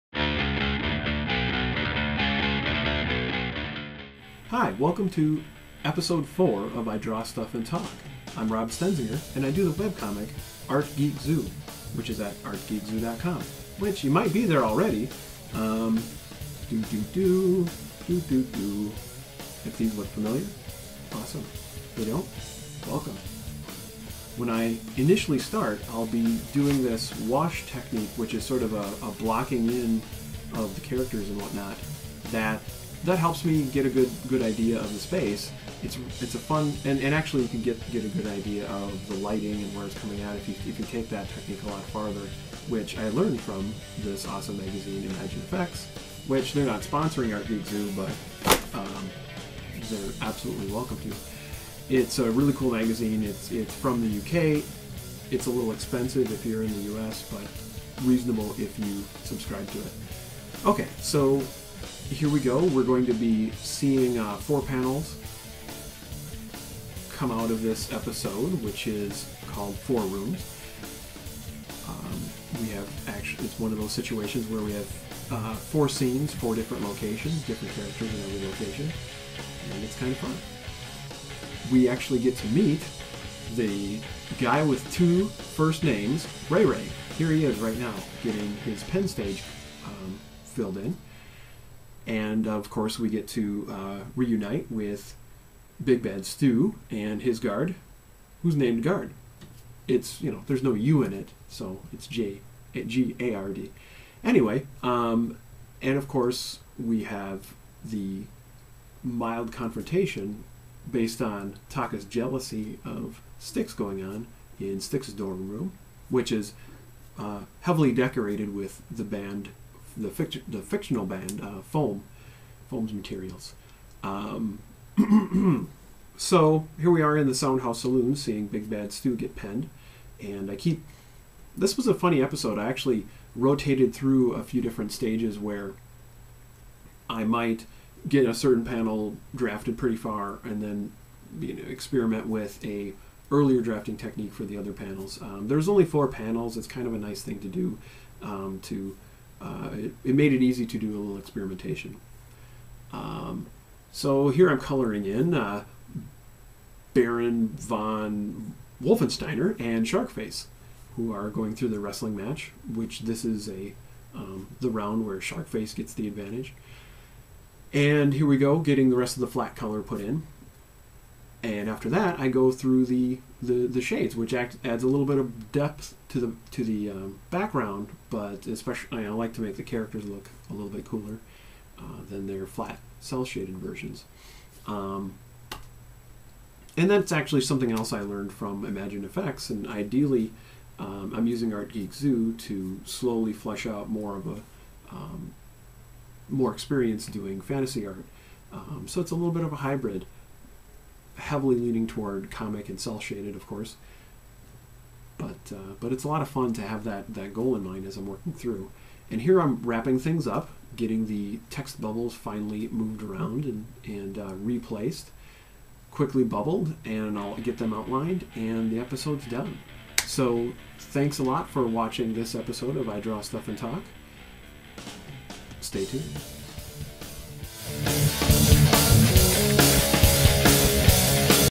AGZ - IDSAT - Ep 4
Speed Drawing of the comic Art Geek Zoo. Hosted by Rob, Music by Rob. In this IDSAT: The picture talks - no more silent corner video, behind the scenes info on the AGZ comic #45 "Four Rooms"